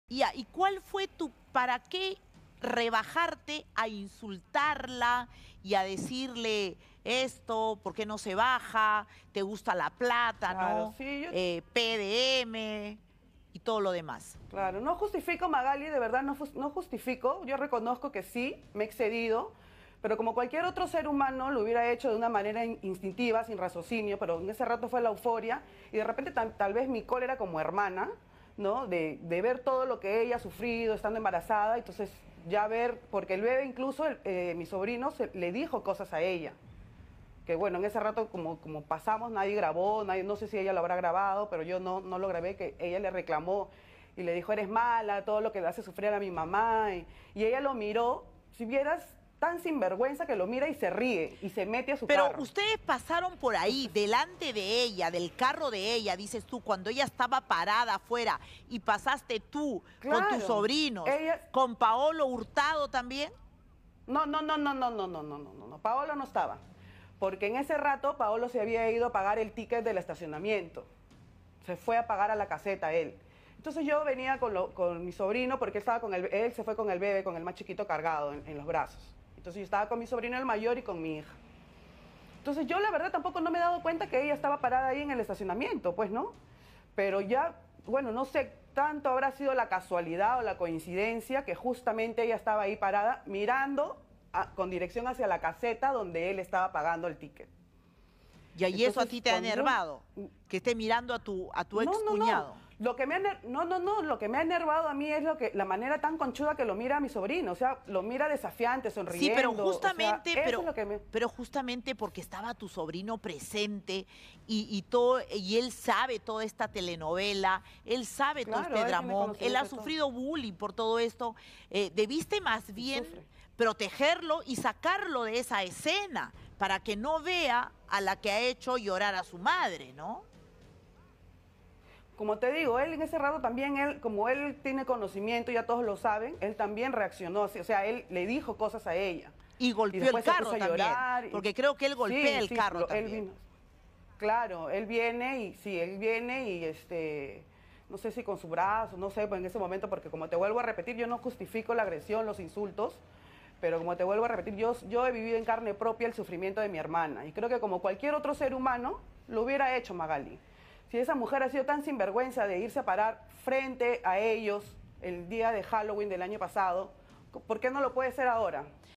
Kiara Fuentes tras insultar a Jossmery Toledo en el Jockey Plaza: "Fue mi cólera como hermana"
Tras todo lo que quedó registrado, la cuñada de Paolo Hurtado reconoce que se excedió.